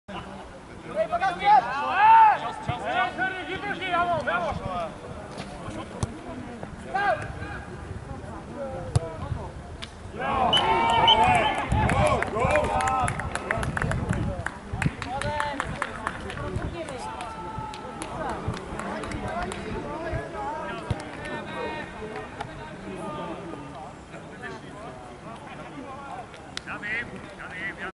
Stod - Chlumčany B 3:5 - Gól Stoda na 1:3
13. kolo Okresního přeboru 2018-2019
Stod - Chlumčany B